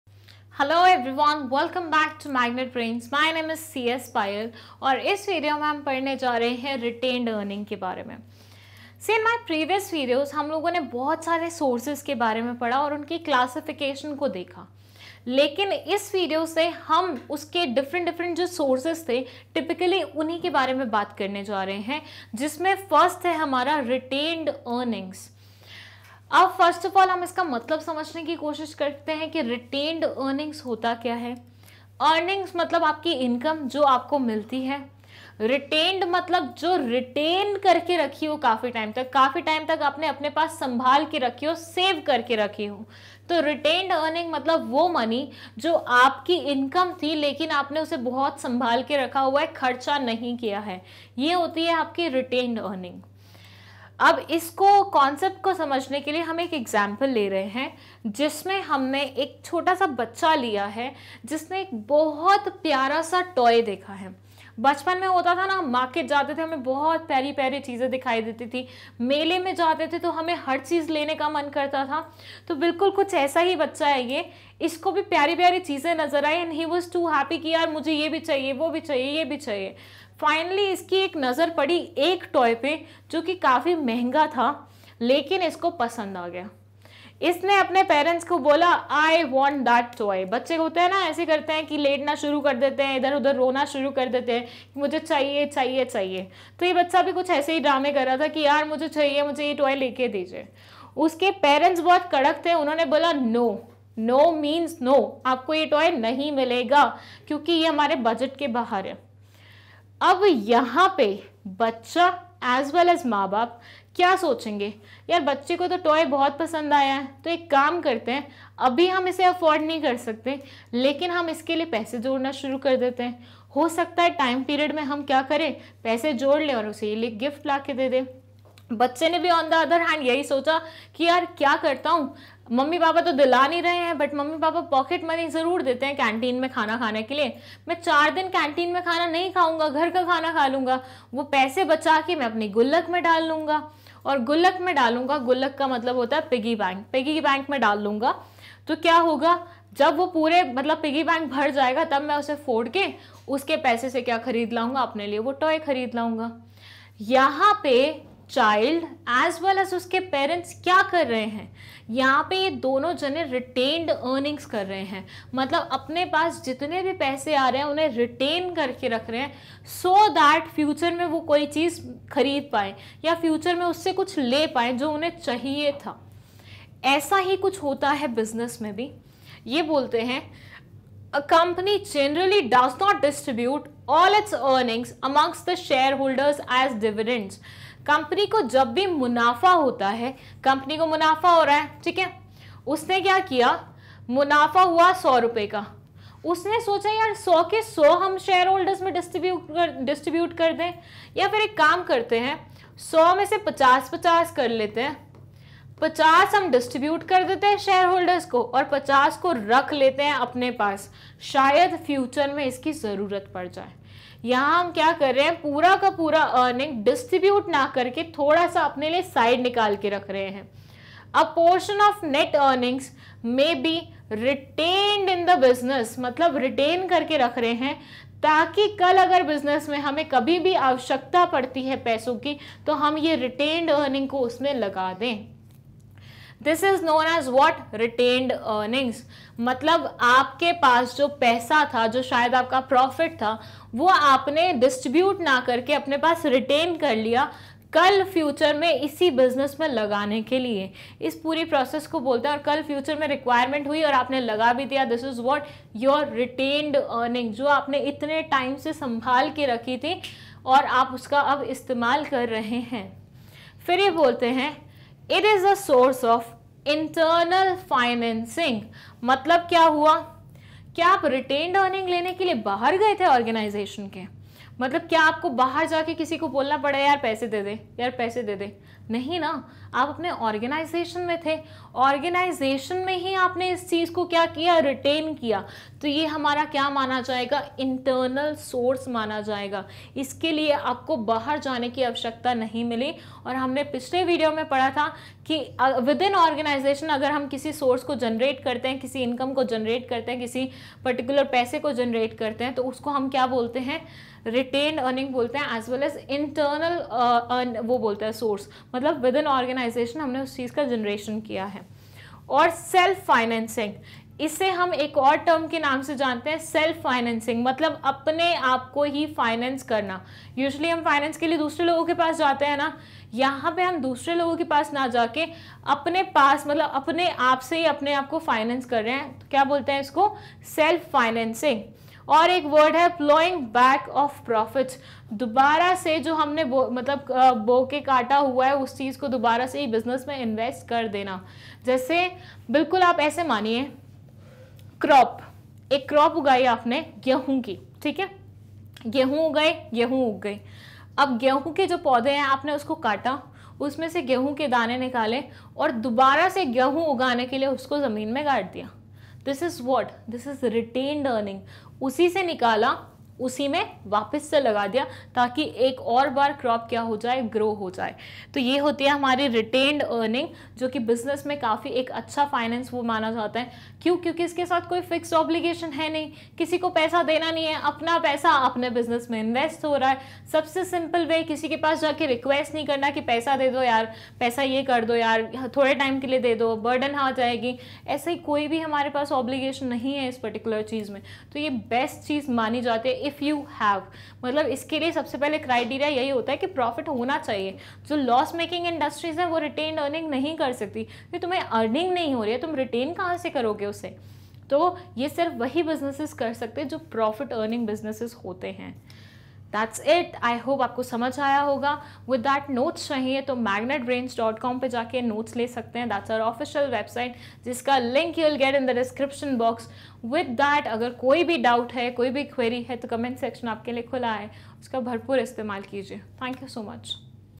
Retained Earnings - Sources of Business Finance | Class 11 Business Studies Chapter 8
✔️ Class: 11th
✔️ Subject: Business Studies
✔️ Chapter: Source of Business Finance (Chapter 8)
✔️ Topic Name: Retained Earnings
.✔️ Topics to be Covered (By Payal Mam): Retained Earnings topic from the chapter Source of Business Finance have been discussed in this video.
00:00 Source of Business Finance Introduction: Retained Earnings
00:06 Retained Earnings